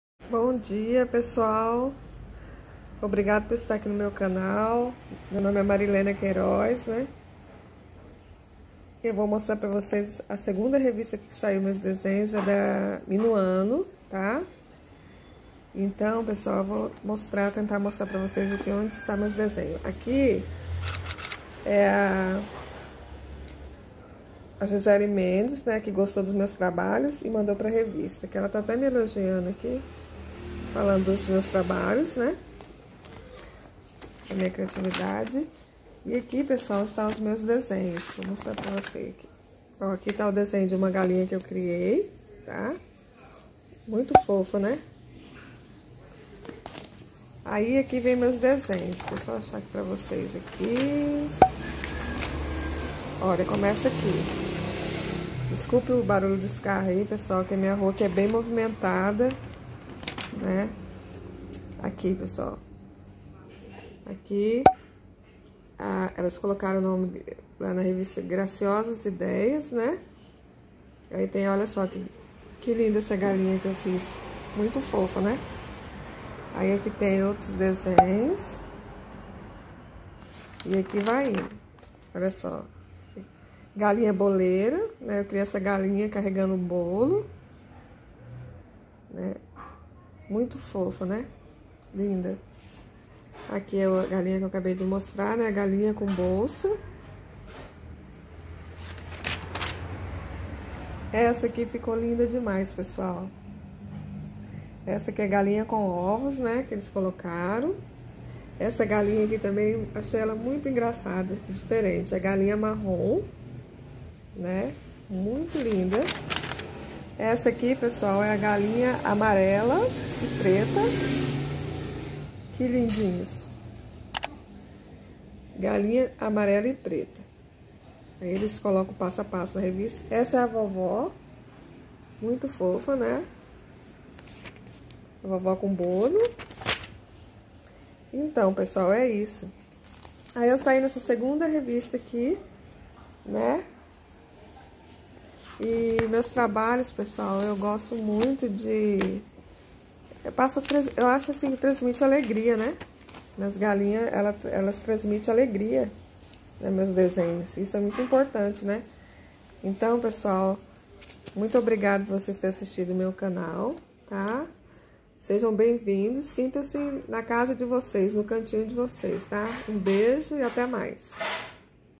A Segunda Revista que meus desenhos saíram
Oi ! amigos essa é a segunda revista que saí...Eu nunca imaginei que iria sair em revistas com meus desenhos. Deus é maravilhoso